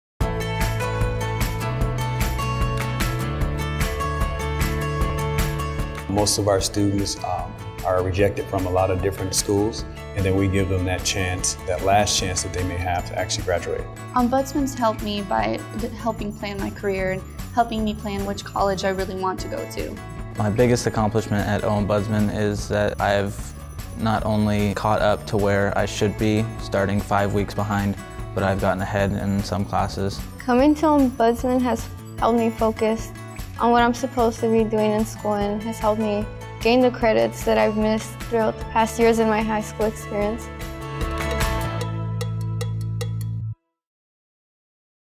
Ombudsman Arizona Charter Schools - Earning Credits and Your Diploma
Students missing the credits needed to graduate can get caught up and earn a real high school diploma at Ombudsman Arizona Charter. With the help of dedicated teachers and support staff, students at Ombudsman get back on-track quickly by focusing on earning only the credits they need.

Ombudsman offers free tuition and open enrollment throughout the school year.